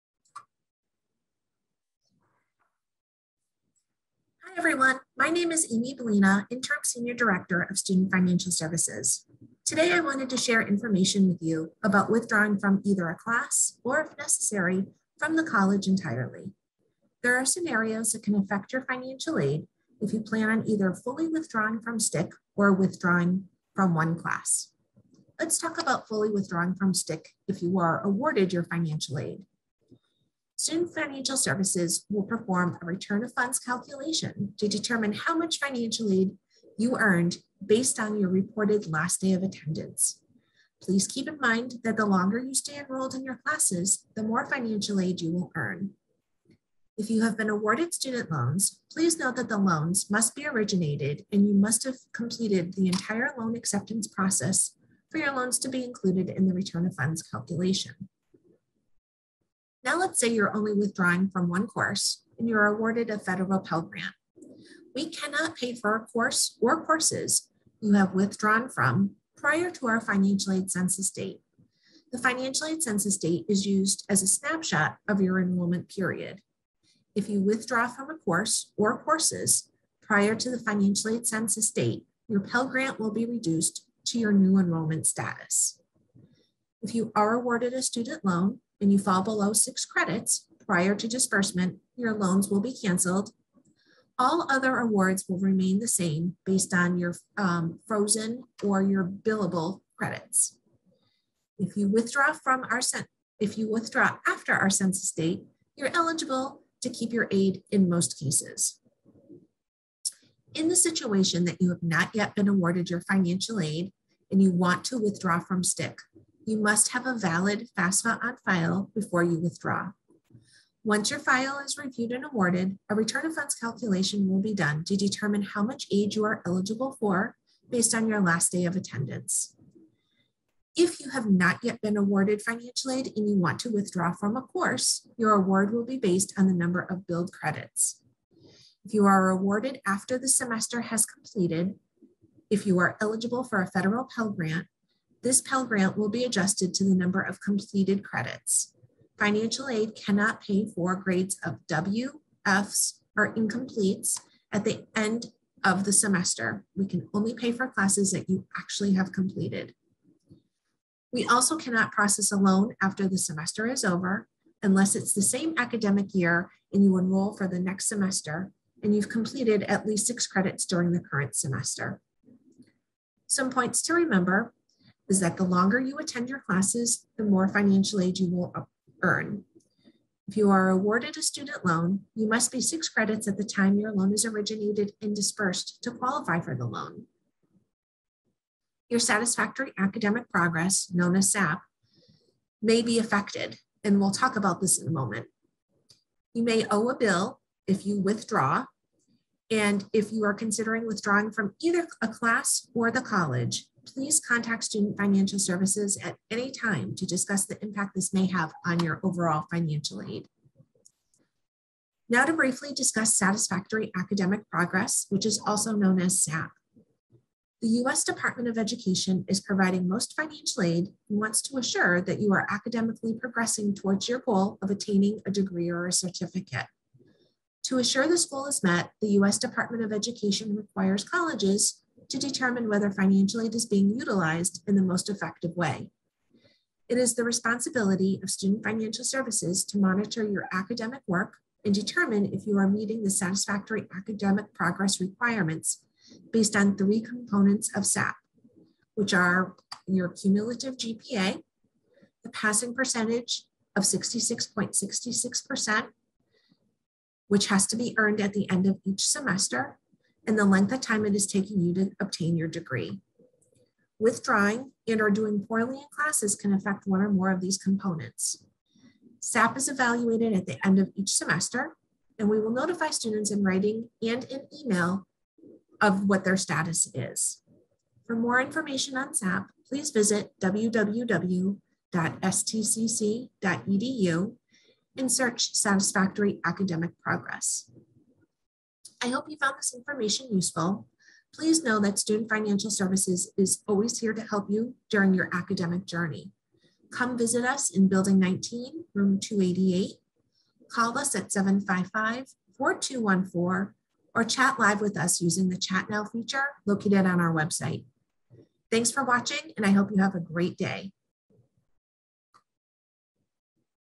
STCC 101: What Happens When You Withdraw?
STCC's interim director of Student Financial Services Amy Belina goes over important topics related to billing and financial aid a student should know when considering withdrawal.